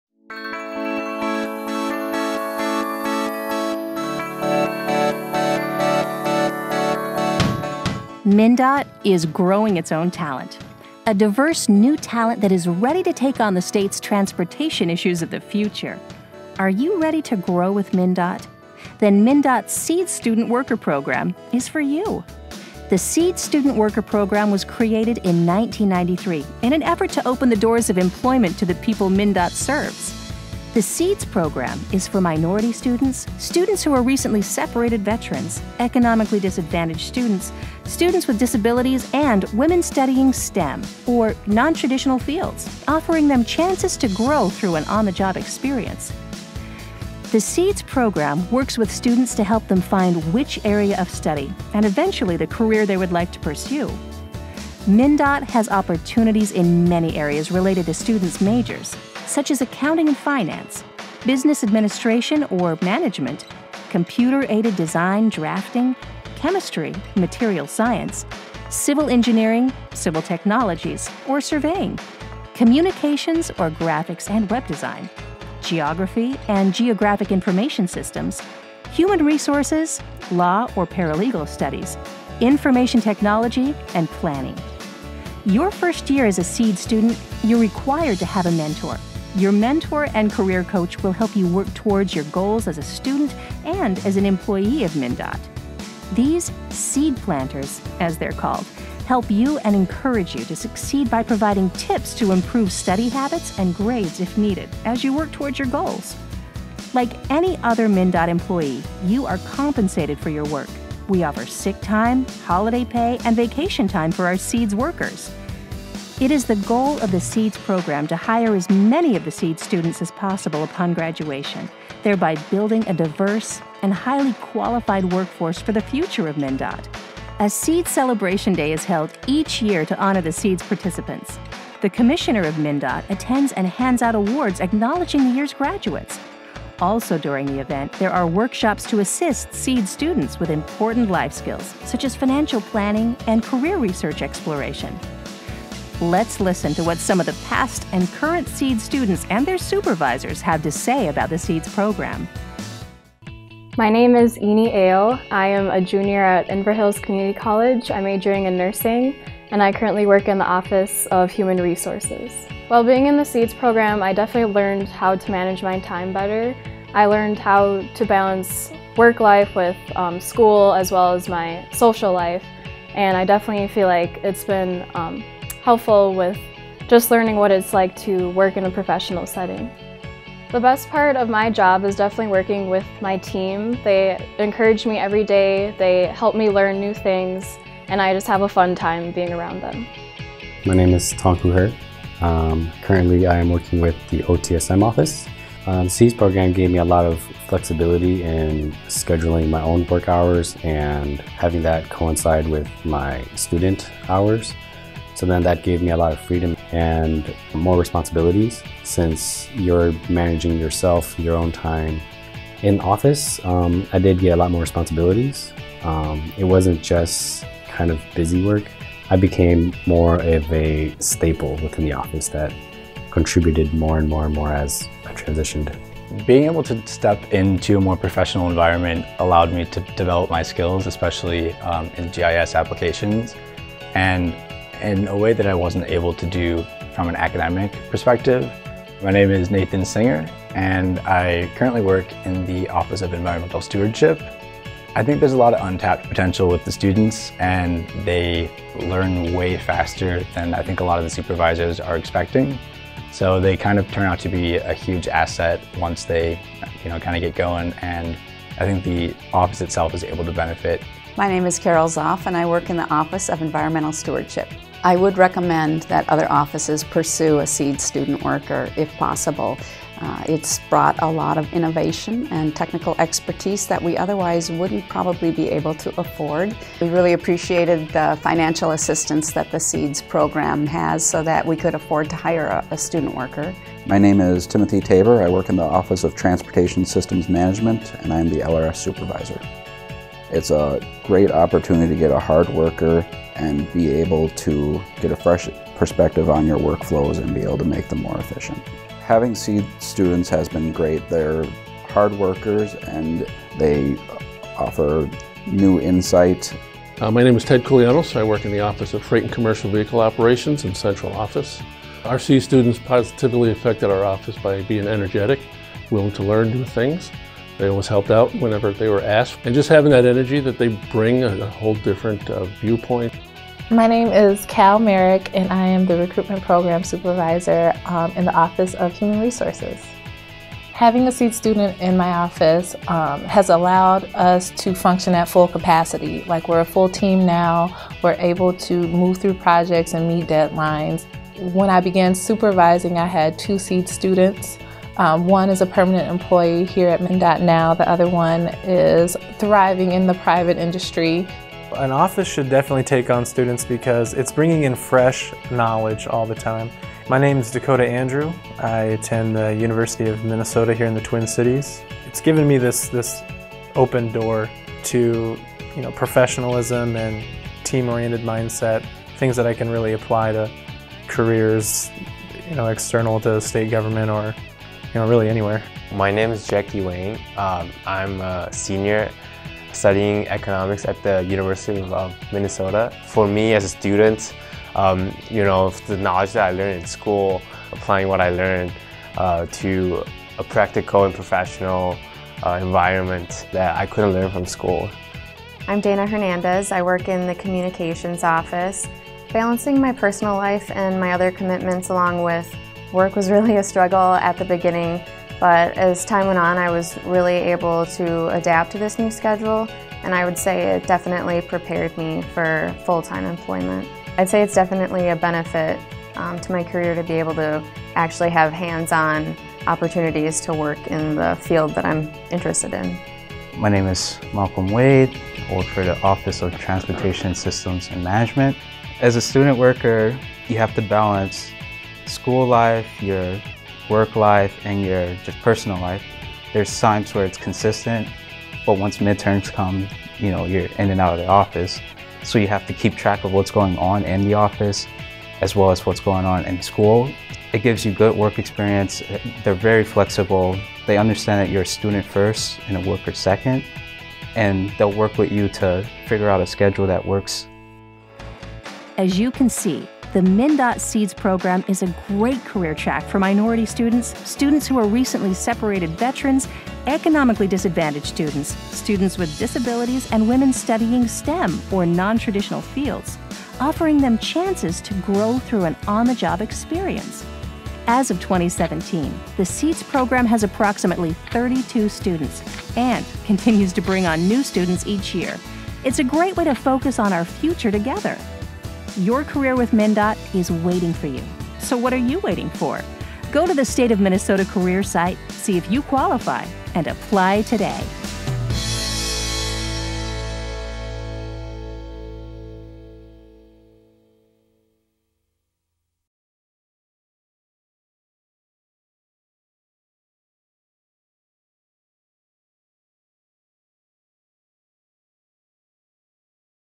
MnDOT - Seeds Student Worker Program
MnDOT's Seeds Program provides paid college-level student worker positions to highly motivated minority or economically disadvantaged college students, students who are recently separated veterans, students with disabilities or female students in STEM or non-traditional fields of study.